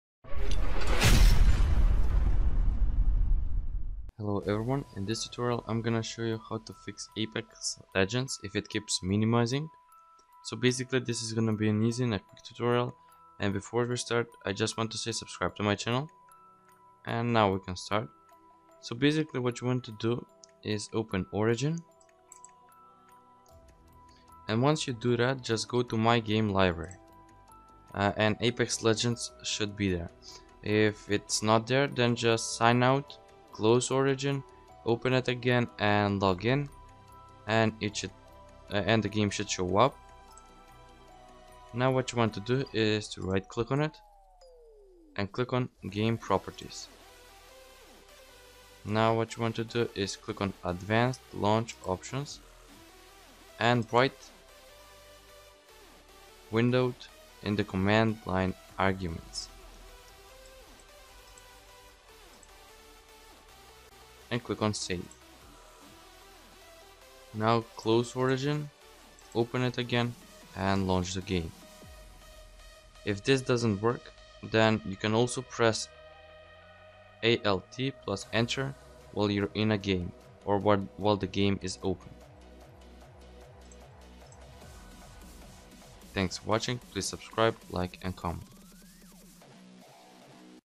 FIX Apex Legends Minimizing !
In this video, I show you how to fix Apex Legends minimizing error.
Lower your ping with NoPing.